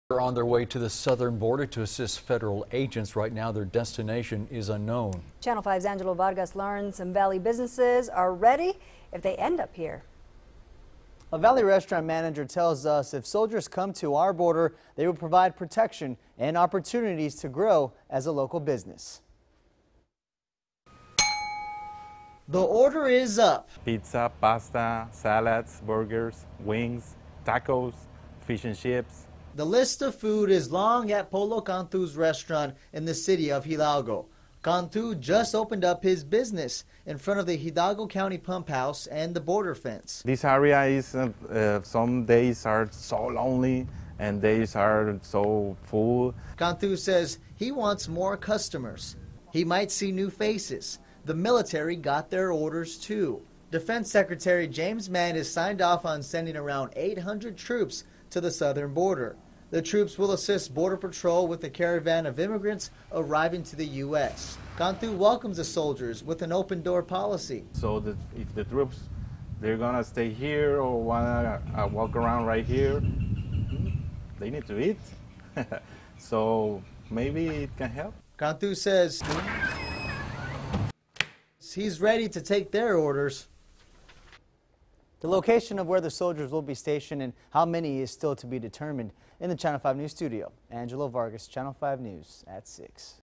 800 Troops Headed to Border to Assist Border Patrol with Migrant Caravan
A local restaurant manager tells CHANNEL 5 NEWS he sees this as an opportunity to grow his business.